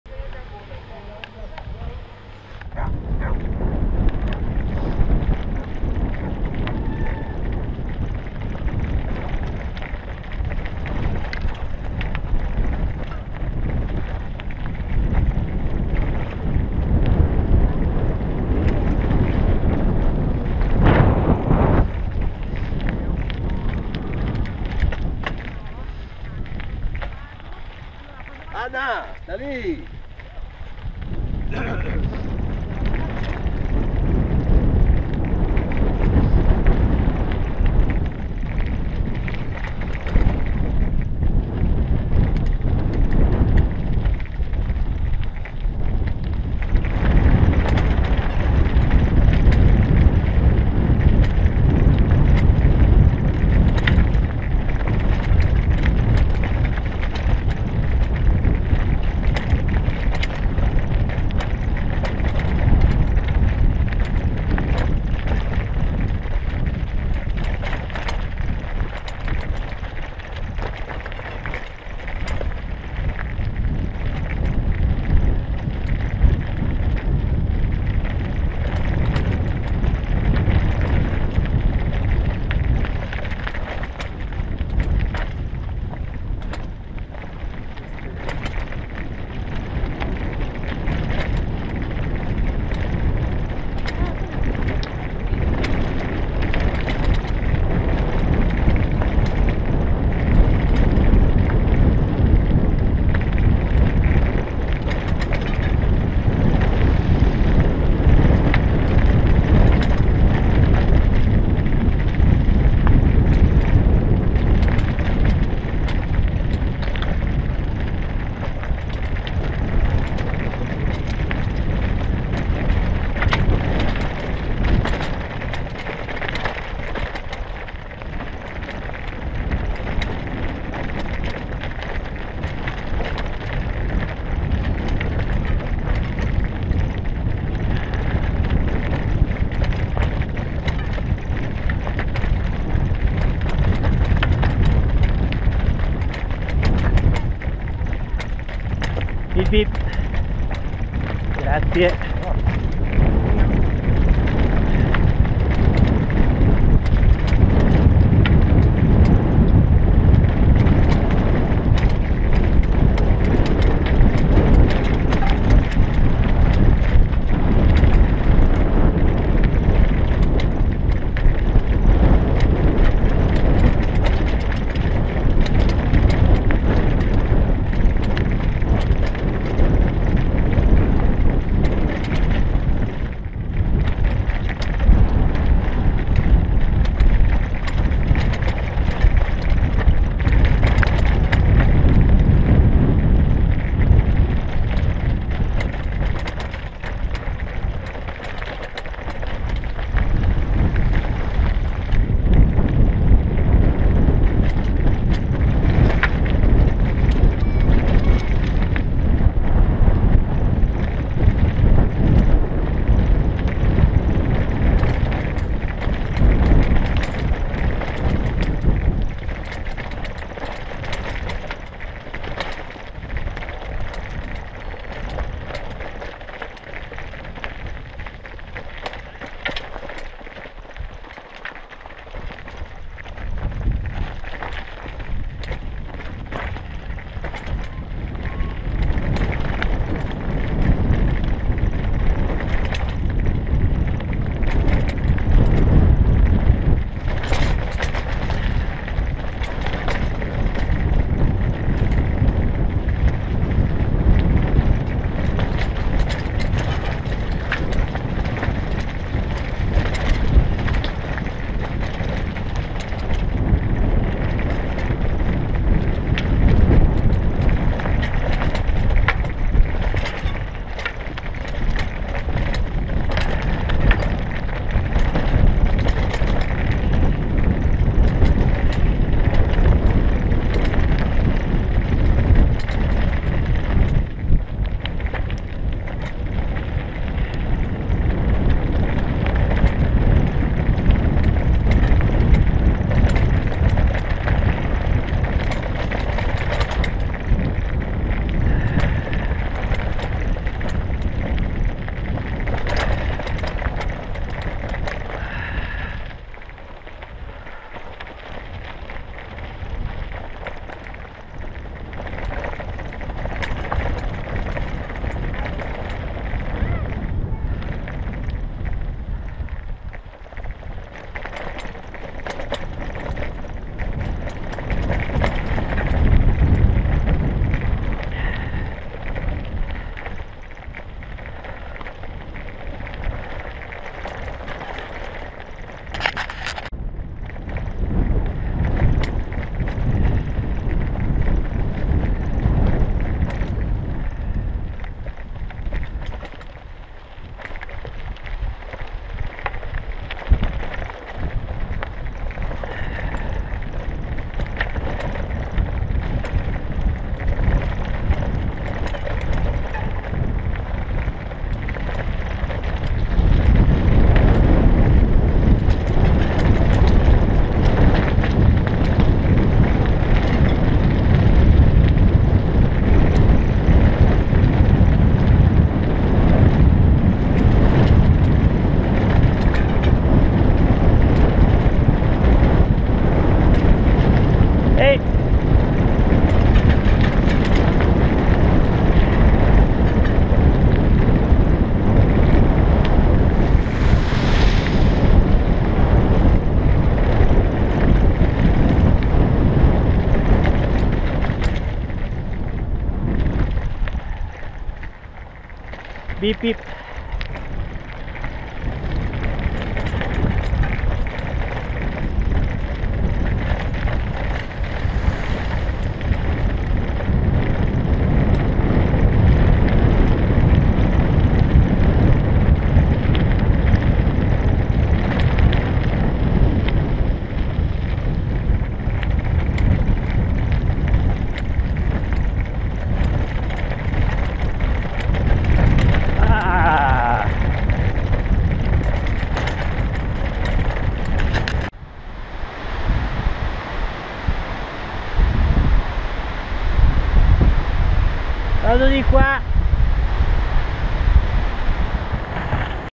Discesa dal Rifugio Bozzi (2478 m.) alle case di Viso, in bici su sentiero di montagna sterrato
L'impegnativa discesa dal Rifugio Bozzi (2478 m.) alle case di Viso, in bici su sentiero di montagna sterrato ha messo a dura prova la mia bici "ibrida" con gomme da strada e meno raggi delle mtb. Impresa di grande soddisfazione ;-)